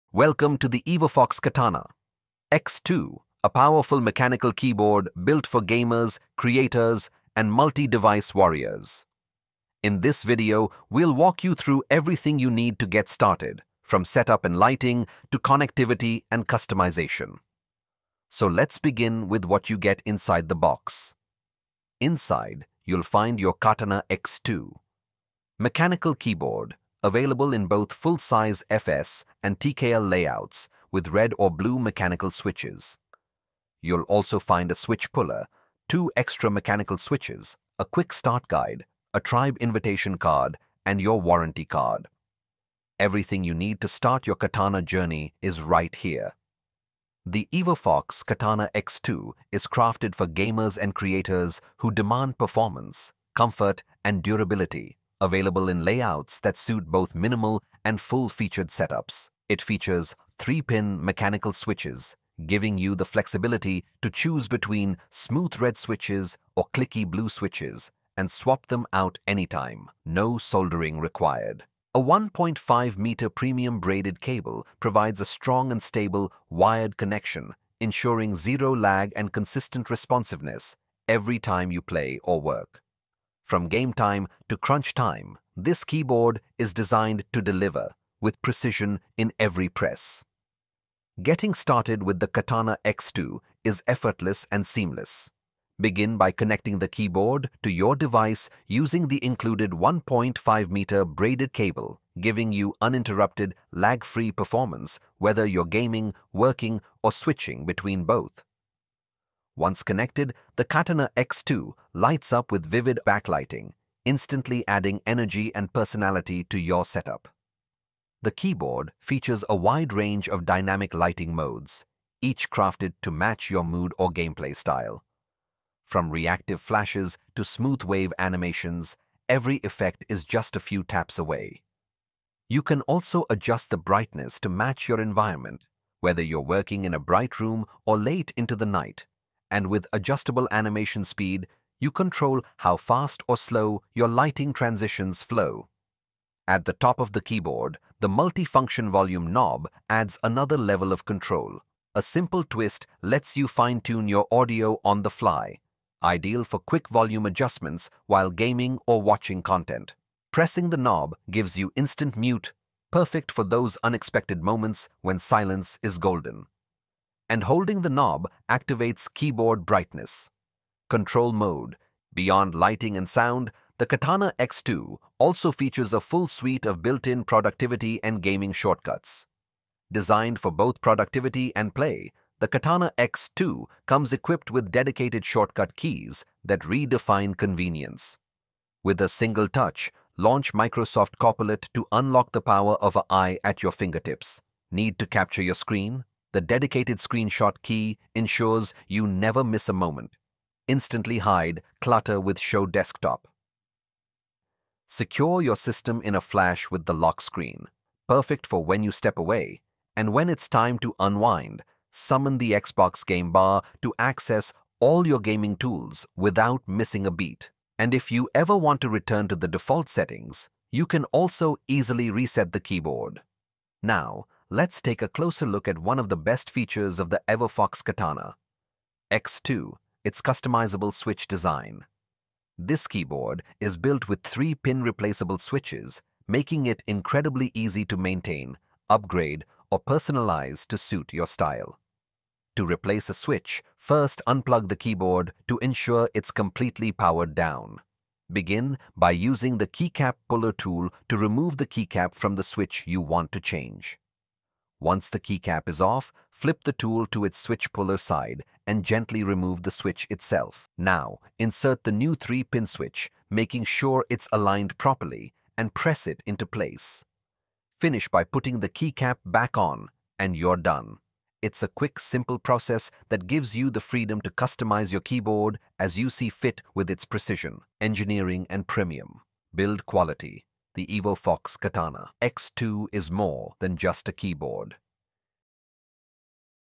EvoFox Katana X2 Mechanical Gaming Keyboard | Complete Setup & Feature Guide
🧰 What’s Inside:
🔌 Setup & Plug-and-Play Instructions
🌈 RGB Lighting Modes & Effects
🎚️ Volume & Brightness Control Knob
🖱️ Shortcut Keys (Copilot, Xbox Game Bar, Screenshot)
🔄 Hot-Swappable 3-Pin Blue Switches
🛠️ Troubleshooting Help

🖥️ Works with: Windows PCs & Laptops
💪 Built to Last: Double Injection Keycaps + Braided Cable